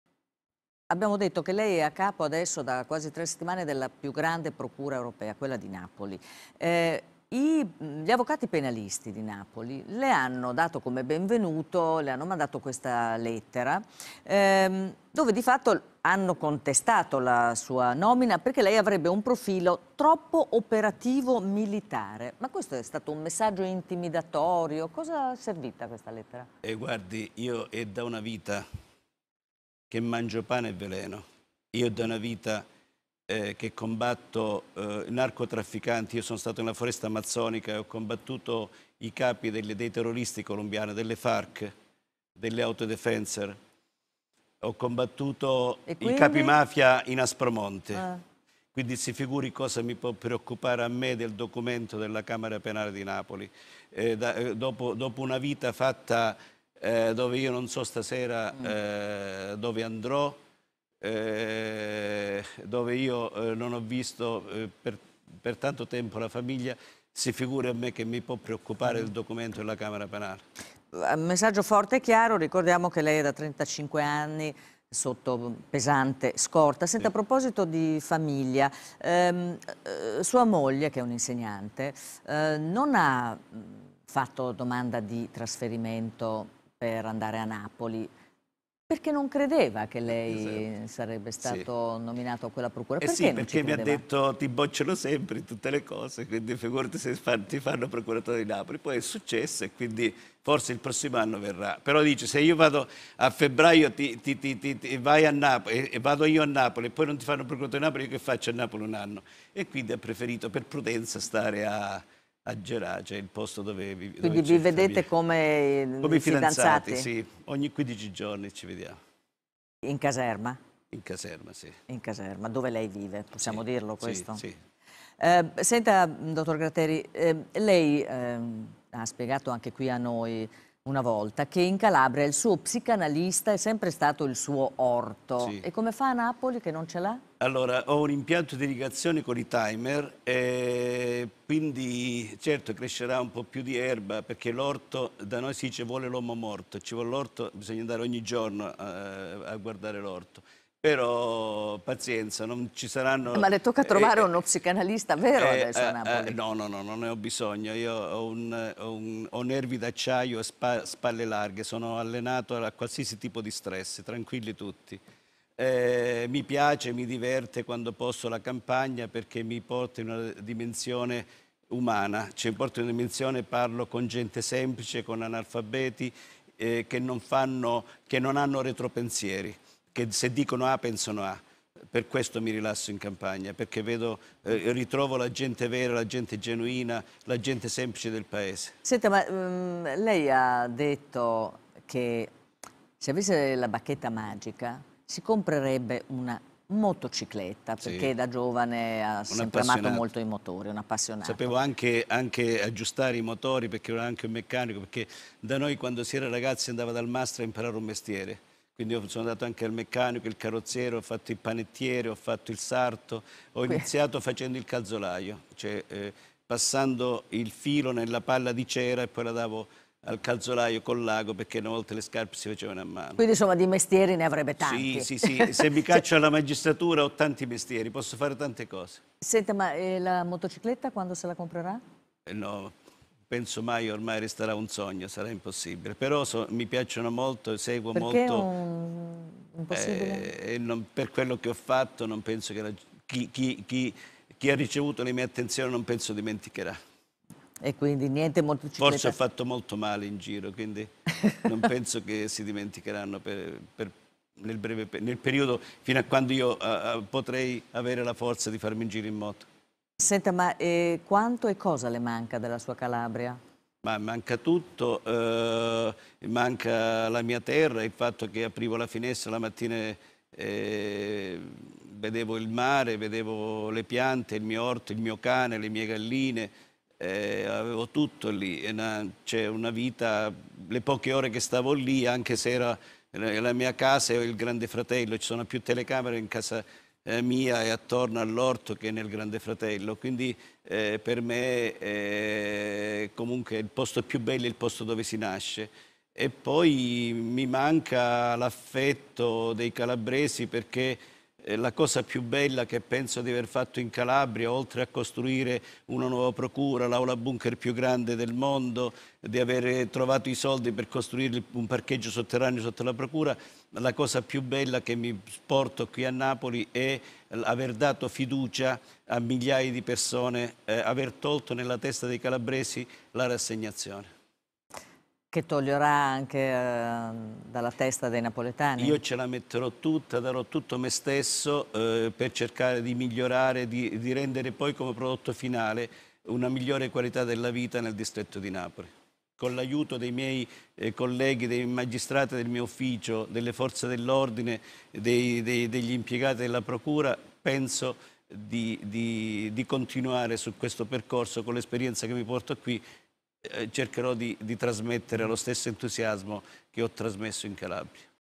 Nicola Gratteri: E' una vita che mangio pane e veleno. La vita nascosta del grande magistrato.
"Dopo una vita fatta dove stasera dove andrò, dove io non ho visto per tanto tempo la famiglia, si figuri se mi preoccupare del documento della camera penale di Napoli" - Così il magistrato risponde alla Gruber dopo che gli avvocati penalisti del tribunale partenopeo gli hanno inviato una lettera di "benvenuto" criticandoli sui suoi metodi ritenuti troppo 'operativi-militari' - Gratteri racconta soprattutto il lato nascosto della sua vita blindata dal 1989 raccontando alcuni aneddoti. E alla fine si augura - "Con l'aiuto dei magistrati e delle forze dell'ordine penso di continuare su questo percorso con l'esperienza che mi porto qui, cercherò di trasmettere lo stesso entusiasmo che ho trasmesso in Calabria" -